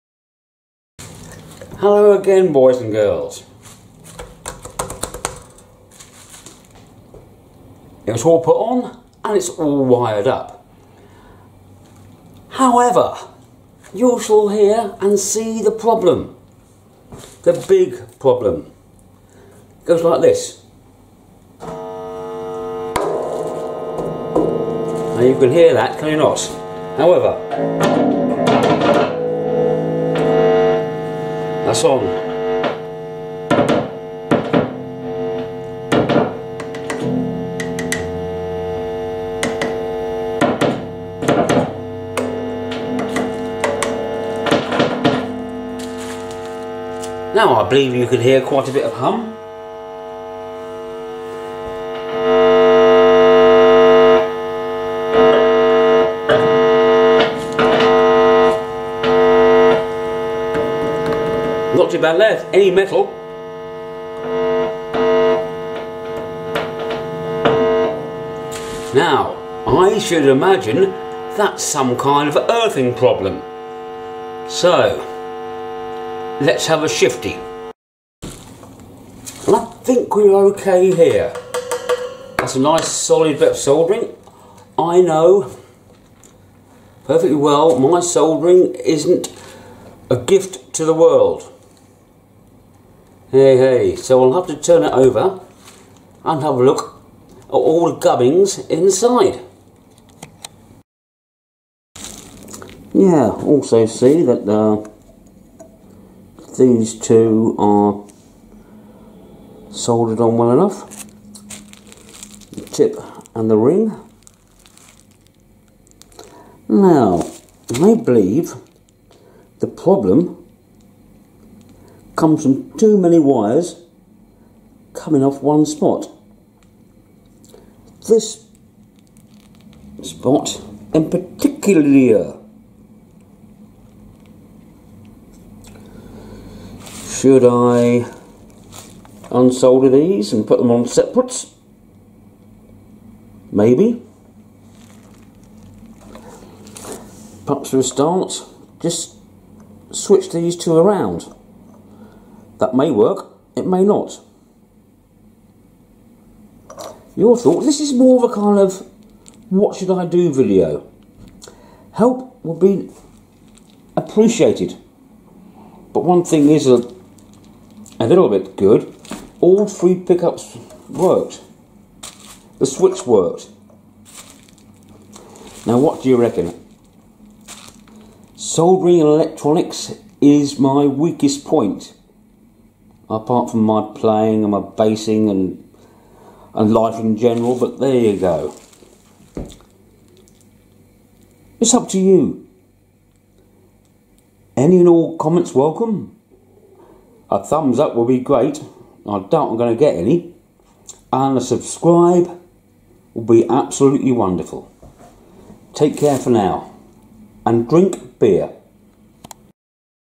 MagiDeal Prewired Strat Pickguard Pt-2, Eathing Problems. Your views and Help?
Well it works, kind of. Any help would be nice,  think I know what's wrong, (yeah right). However you my have a differing point.